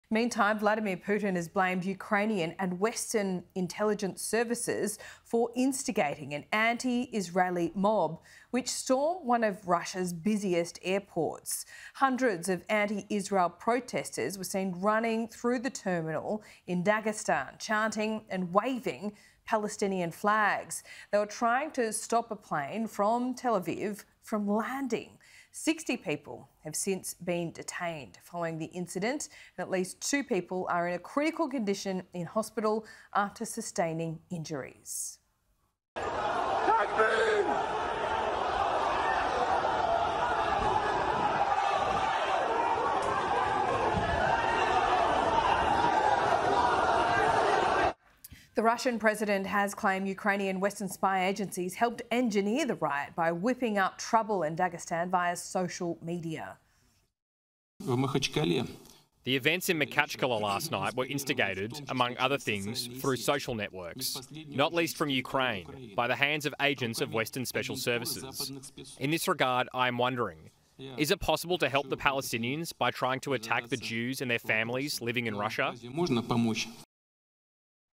Putin blames Ukraine and the West for antisemitic riot at Dagestan airport
Vladimir Putin has blamed Ukrainian and Western intelligence services for instigating an anti-Israeli riot at one of Russia's busiest airports.

Hundreds of anti-Israel protestors were seen running through the terminal in Dagestan, chanting and waving Palestinian flags.

Protesters were trying to stop a plane from Tel Aviv landing.

Around 60 people have since been detained following the incident, and at least two people are in a critical condition in hospital after sustaining injuries.

The Russian President has claimed Ukrainian and Western spy agencies helped engineer the riot, by whipping up trouble in Dagestan via social media.